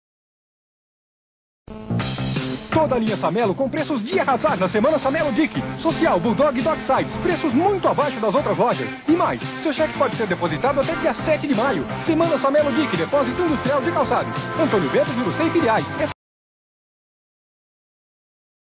DIC - Anos 90
Muitos paulistas vão lembrar da era de ouro da DIC. Várias lojas espalhadas por São Paulo. O Depósito Industrial de Calçados marcou a lembrança de muitas crianças com a distribuição das bolas coloridas. Confira uma propaganda da rede de lojas, em meados dos anos 90.